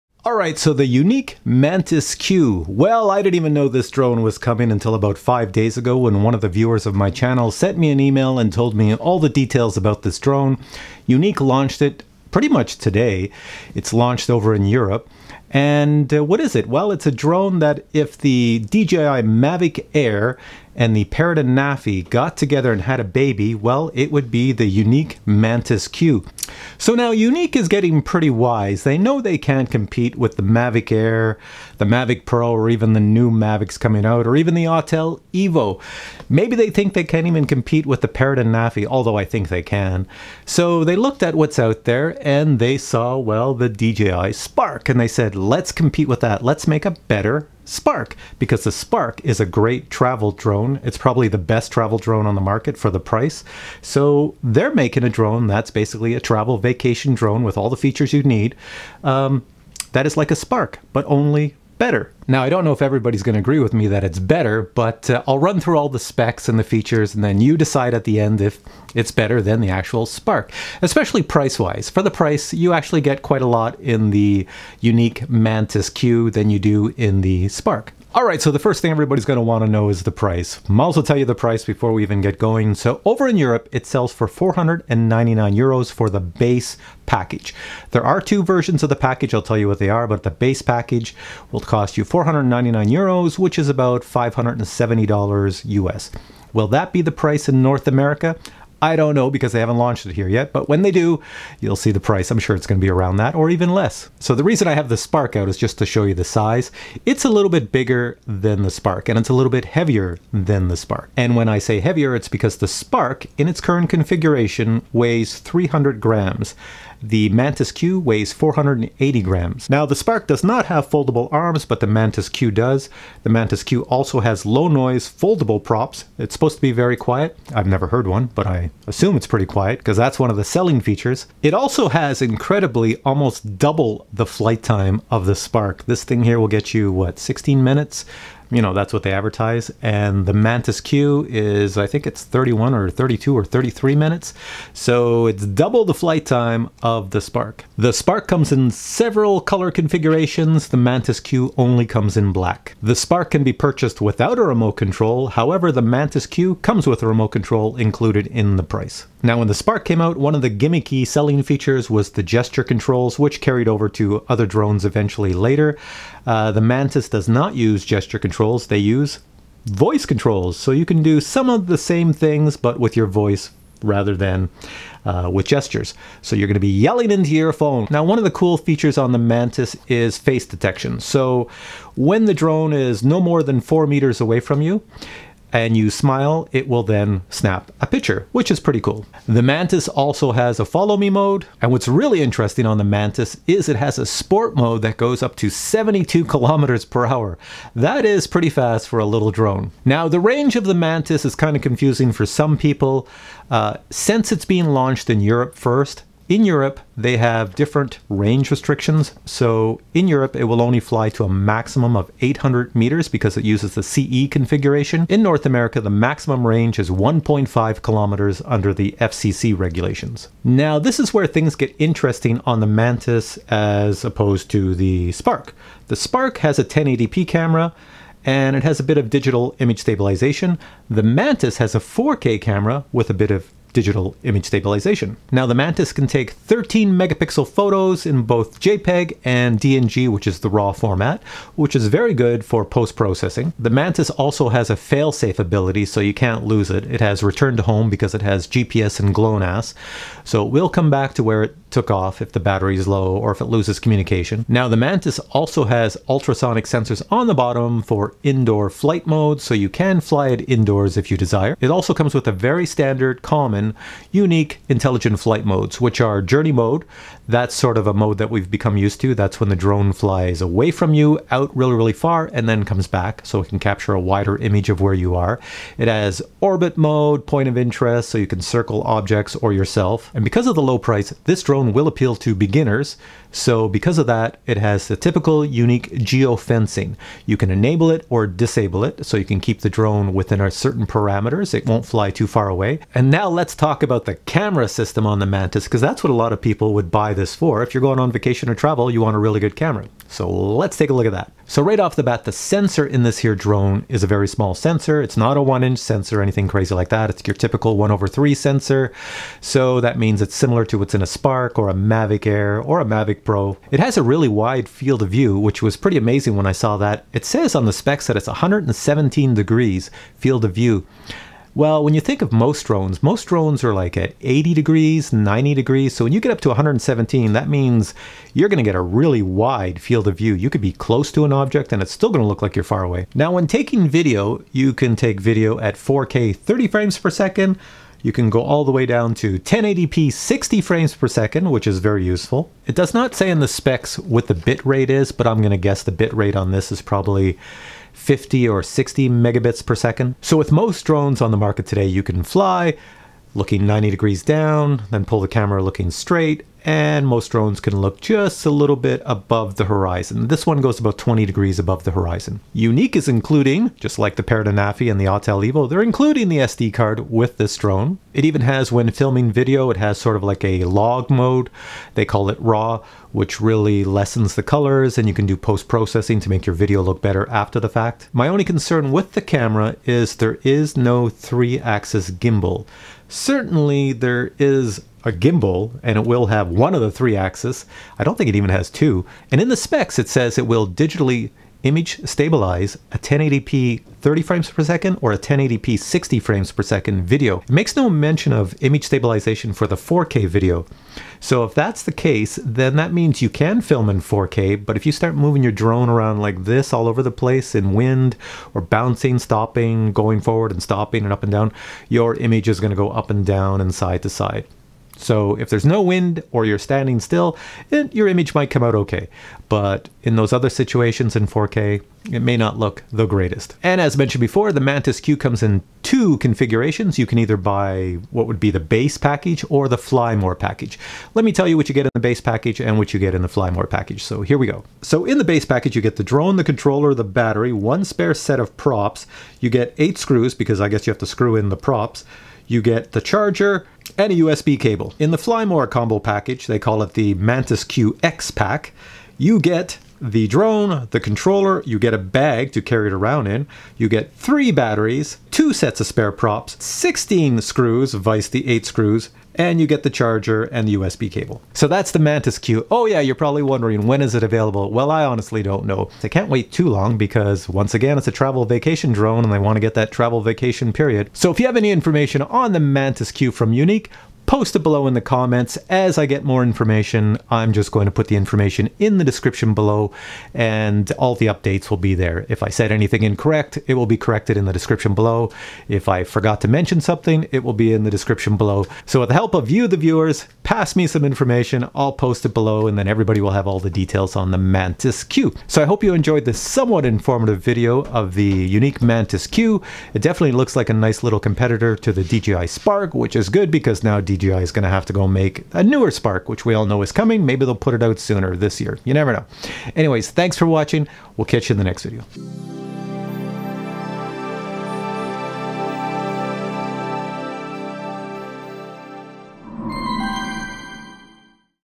YUNEEC MANTIS Q - The New Travel Drone - Better than the DJI Spark?
Yuneec quietly launched the MANTIS Q which is a BIG upgrade to the Yuneec Breeze.  It is in direct competition with the DJI Spark.  NOTE:  The MANTIS Q does NOT have OBSTACLE AVOIDANCE.  The DJI Spark does.  Also, the $499 Euro price includes VAT which does not exist in North America.  The MANTIS Q will sell for $499 US before taxes.

Want a Captain Drone T-Shirt or Coffee Mug?

BELOW IS THE GEAR I USED TO CREATE THIS VIDEO:
CAMERA: Panasonic 4K Camcorder
LIGHTING: Studio Softbox